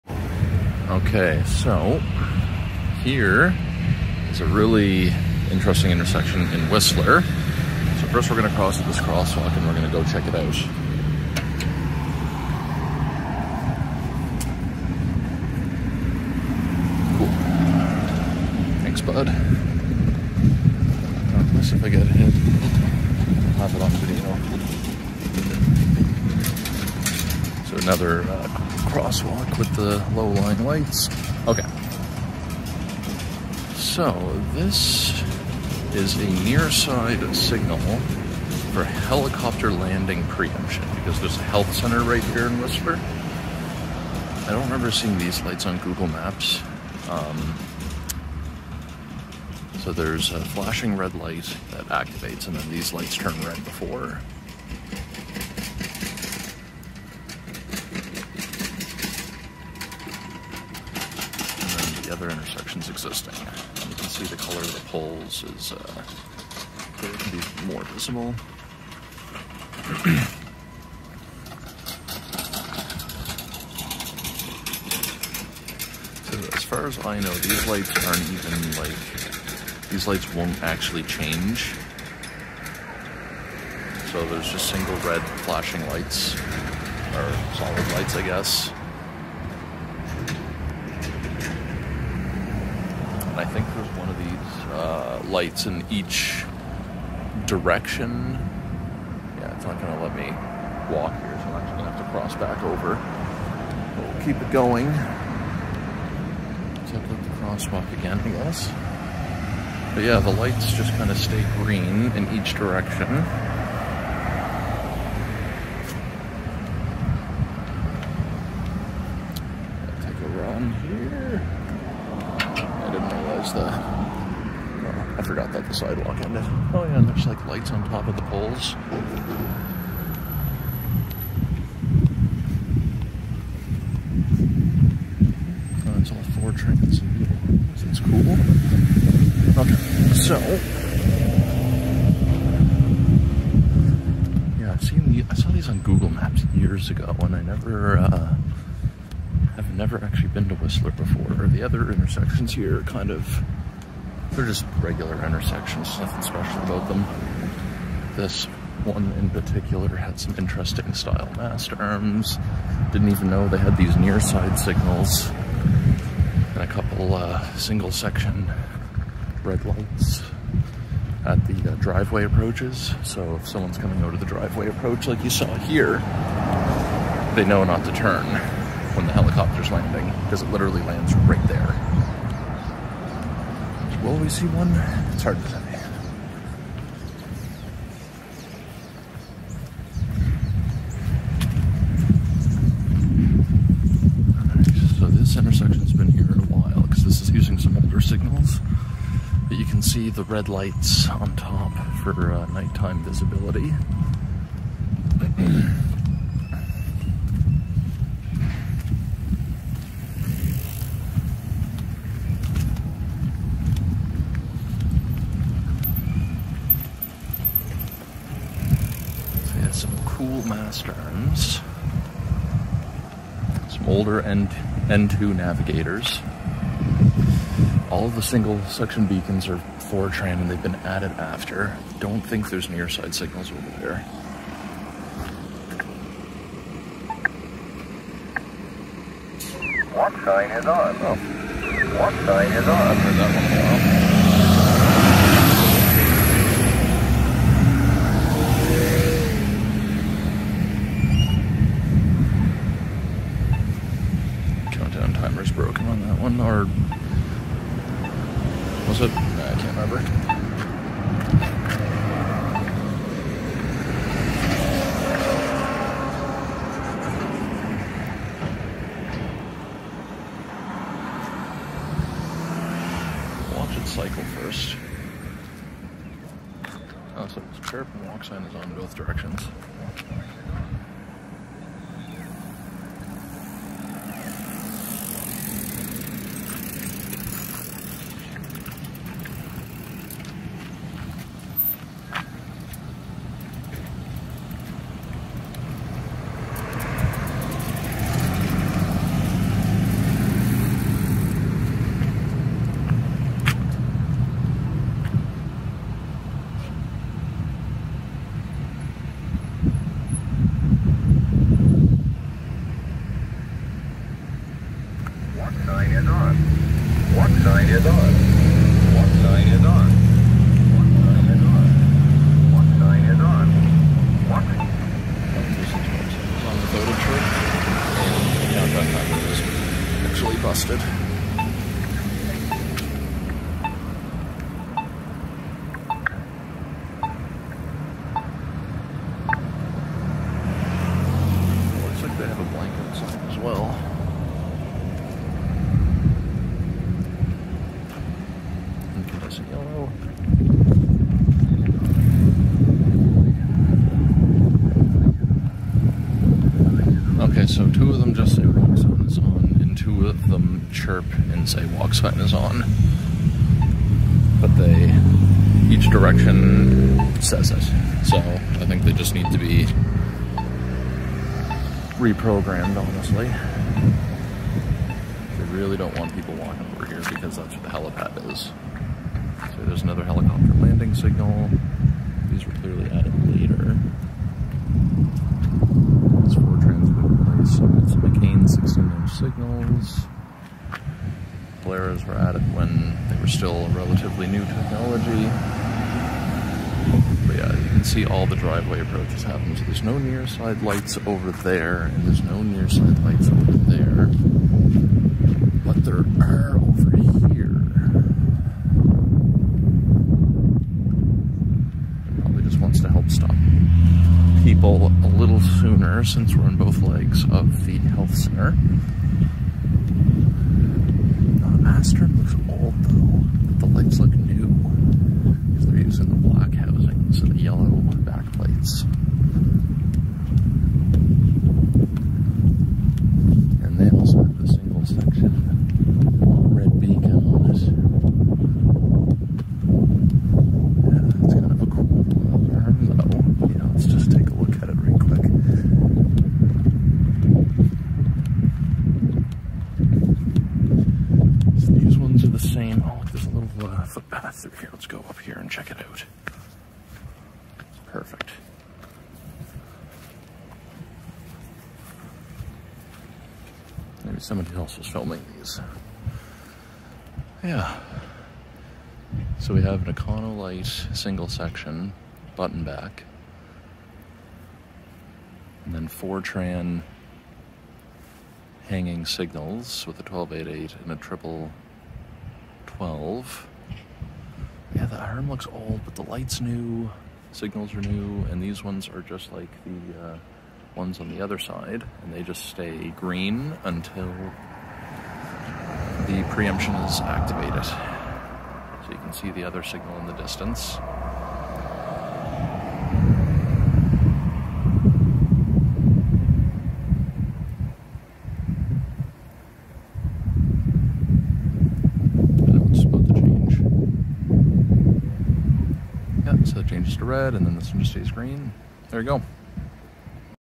Traffic Signals with Helicopter Preemption in Whistler
This is my last BC video from this collection. I figured I'd save this one for last because it's very interesting. There is a hospital at this intersection here in Whistler and from two approaches, there are queue cutter signals that are always green except when a helicopter is landing. When a helicopter is taking off or landing, the preemption is triggered and I believe the queue cutter signals turn red and the intersection is cleared out. I'm not sure if it rests in red or if it dwells certain phases but holds or flashes the red beacon on the mast arms. Either way it was a neat intersection to come across and the mast arms are neat looking.